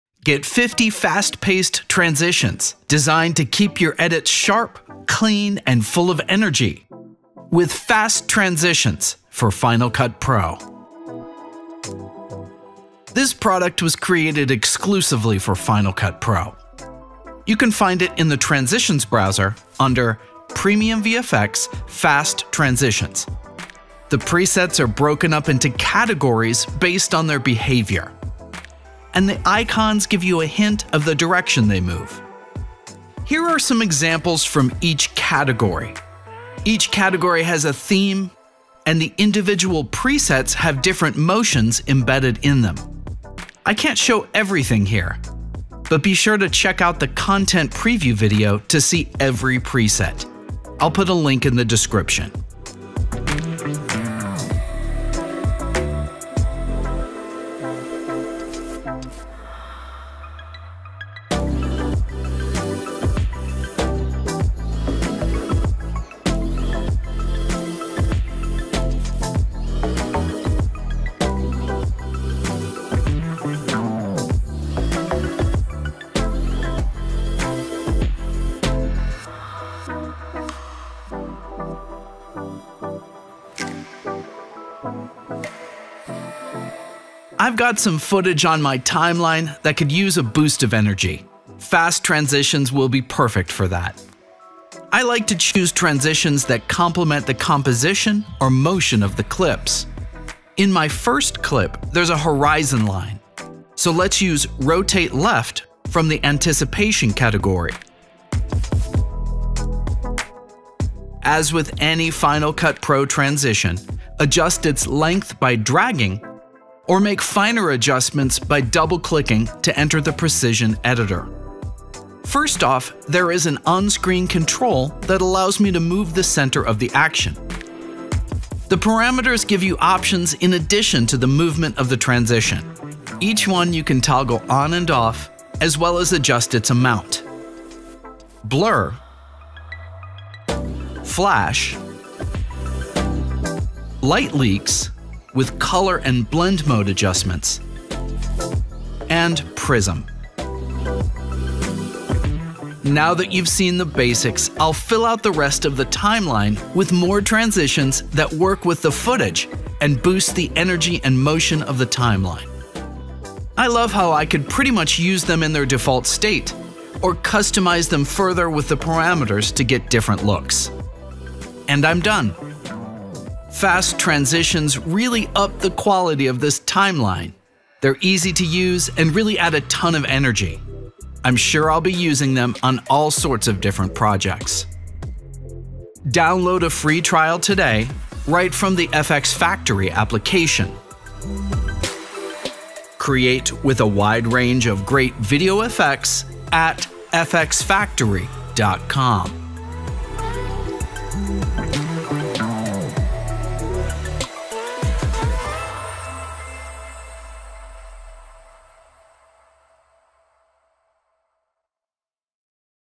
First Look at Fast Transitions for Final Cut Pro
Give your edits a burst of speed and energy with Fast Transitions for Final Cut Pro.

This collection includes 50 fast-paced transitions, built to keep your timeline sharp, clean, and full of motion.

Transitions are organized by behavior and direction, making it easy to find the perfect one for your edit.

Customize each preset with options like blur, flash, light leaks, and prism effects to match your creative style.

Use them right out of the box, or tweak parameters for a unique look.

Fast Transitions are perfect for music videos, social content, reels, trailers, and high-energy edits of all kinds.

CHAPTERS:

00:00 Intro
00:12 Final Cut Pro Tutorial
00:32 Preset Categories
02:52 Final Examples
03:05 How to Download
03:12 Learn More!